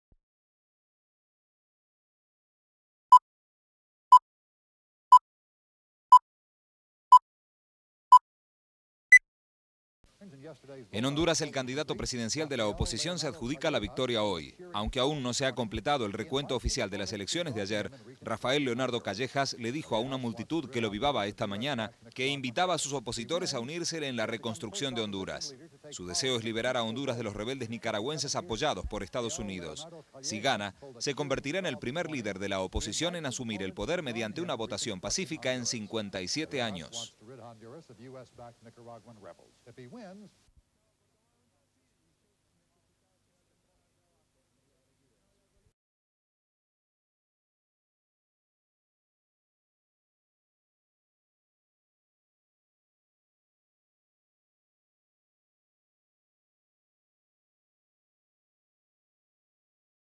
DiFilm - Rafael Leonardo Callejas gana las elecciones en Honduras 1989
En Honduras el candidato presidencial de la oposición se adjudica la victoria aunque aun no se ha completado el recuento oficial de las elecciones de ayer. Rafael Leonardo Callejas le dijo a una multitud que lo avivaba esta mañana que invitaba a sus opositores a unírsele en la construcción de Honduras. Su deseo es liberar a Honduras de los rebeldes nicaragüenses apoyados por Estados Unidos, si gana se convertirá en el primer líder de la oposición en asumir el poder mediante una votación pacifica en 57 años. Insert de la gente votando en Tegucigalpa.
Fecha: 26/11/1989
Duración: 35 segundos
Código: UG-1539